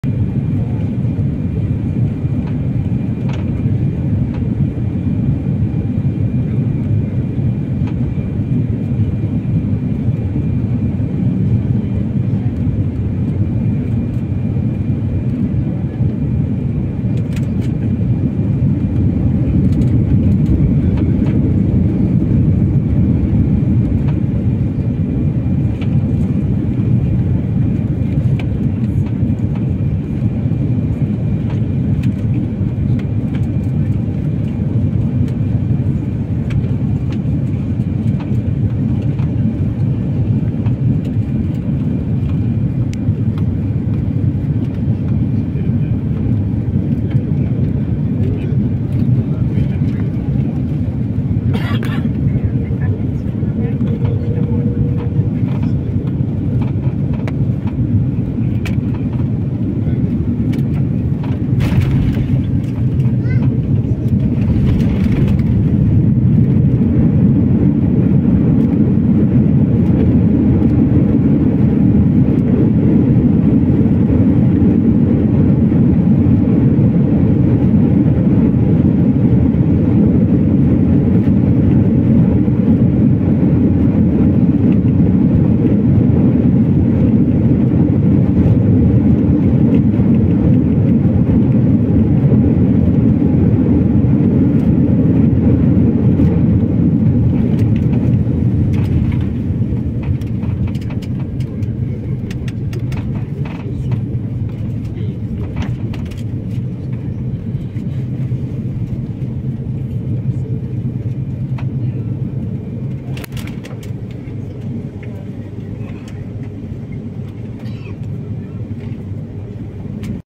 Brussels Airlines Brussels (BRU) to Ouagadougou Flight SN255 A330-300 - Landing
Flight : Brussels Airlines SN255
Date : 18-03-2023
Phase : Landing
Departure Airport : Brussels (BRU)
Arrival Airport : Ouagadougou (OUA)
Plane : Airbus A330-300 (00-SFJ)